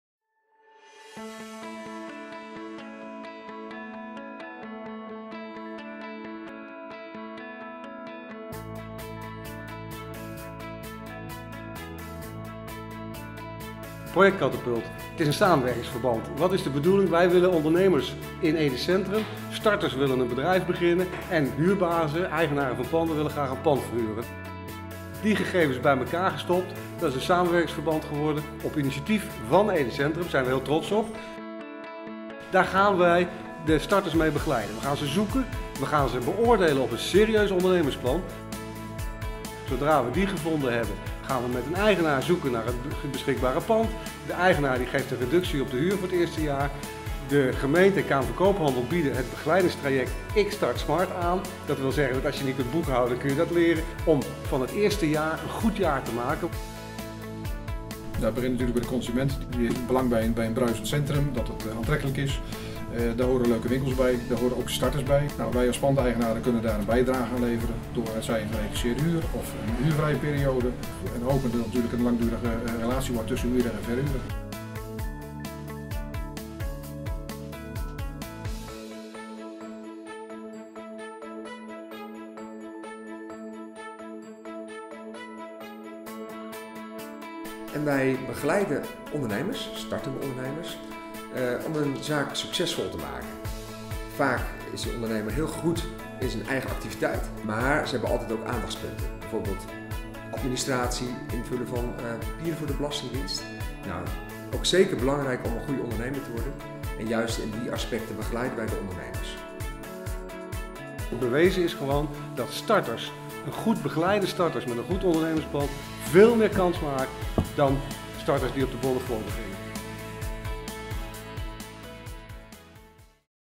Project Katapult in Ede Centrum
Ondernemers opgelet! Dit is je kans om een vliegende start te maken in Ede Centrum.
Het nieuwe project ‘Katapult’ helpt jou om je ondernemingsplan werkelijkheid te maken. Katapult is een initiatief van Stichting Binnenstadsmanagement Ede (SBE). De deelnemende partijen zijn makelaars/vastgoedeigenaren, IkStartSmart (KvK) en de gemeente Ede.